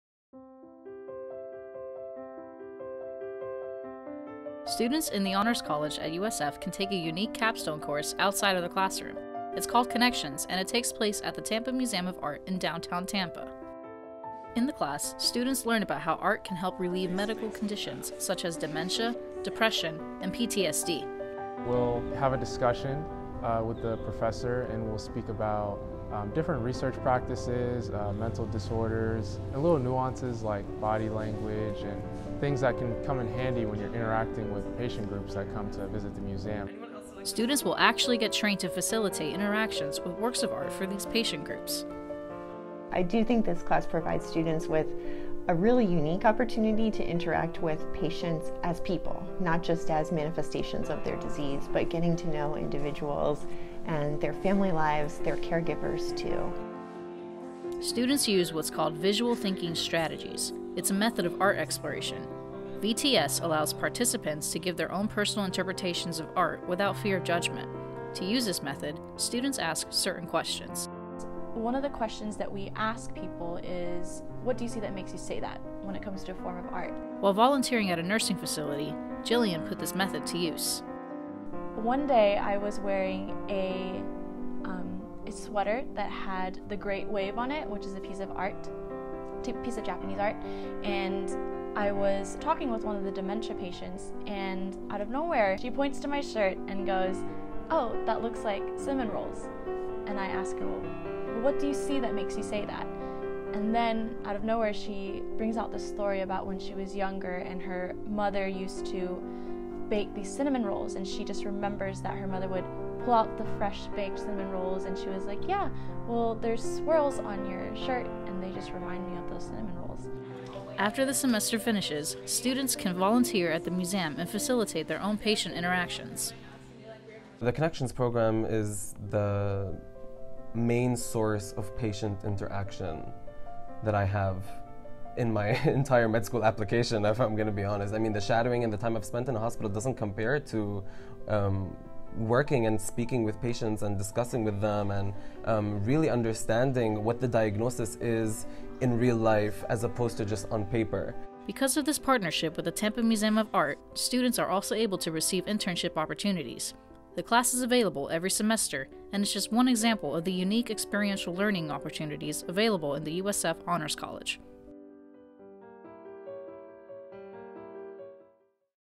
Tampa Museum of Art | USF Honors College Capstone Course
Since 2015, the USF Honors College has participated in a partnership with the Tampa Museum of Art that provides research opportunities for students as well as a vital service to the community. In a Capstone course called Connections, Honors students learn about medical conditions such as dementia, depression, and PTSD and are trained to facilitate interactions with works of art for patient groups dealing with these diagnoses. Upon completion of the course, students facilitate special tours at the Tampa Museum of Art on a weekly basis. The program is based on the Meet Me at MOMA Alzheimer's Project, which strove to make art accessible to people with dementia, and has been highly successful and lauded by healthcare professionals, museum staff, and participants alike. Students in the Visual Thinking Strategies method of art exploration, which allows participants to give their own personal interpretations of works of art without fear of judgment or failure. Particularly in people with Alzheimer's, depression, and PTSD, this method has been found to help patients access and express memories, practice or regain their communication skills, externalize emotions, relieve stress and anxiety, and promote positive emotions. Through this community partnership, students also participate in furthering the research in the areas of art, medicine, and mental health. The partnership with the Tampa Museum of Art has also grown to include internship opportunities for students as well.